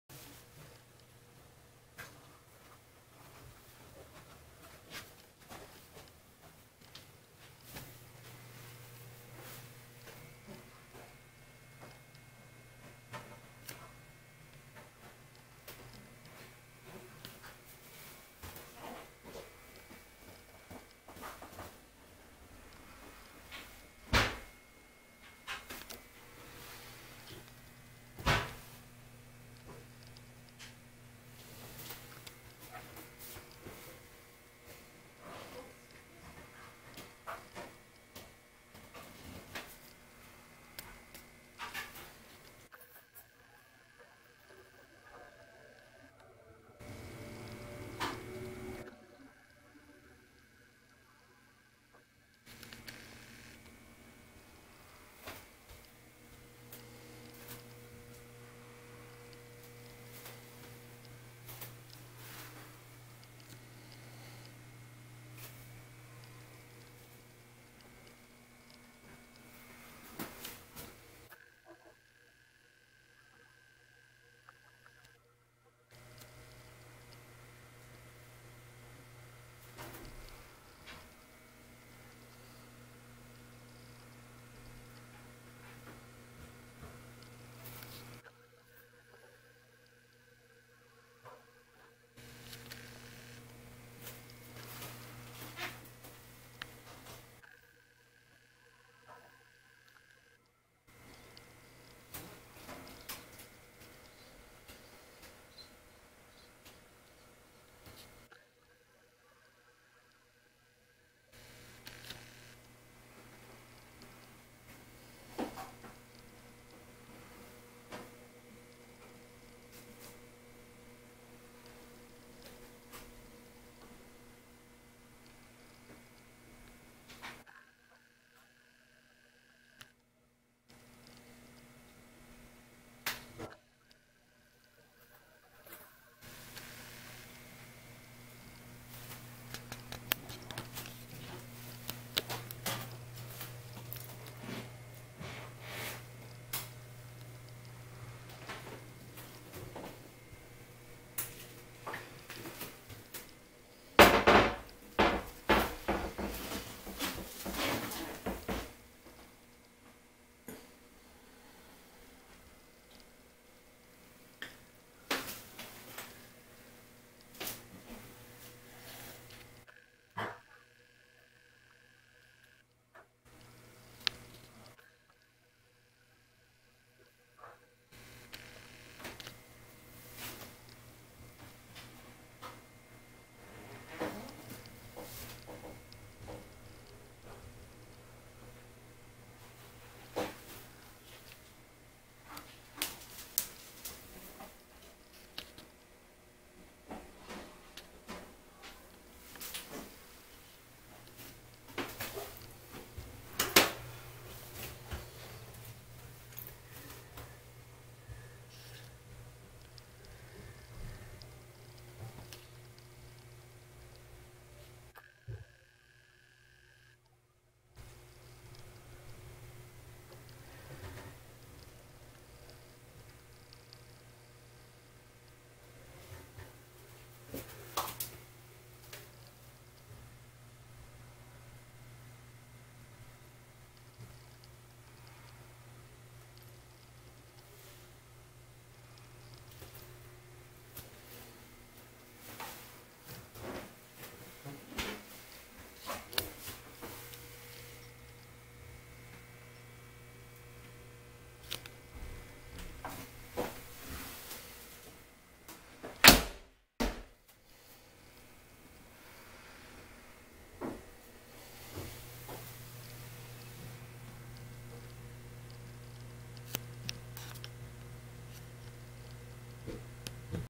Mural with mermaid on a boat!
I just painted this mural in the master head on a 58 foot Hatteras yacht. It was my first mural on a boat, and was a great experience.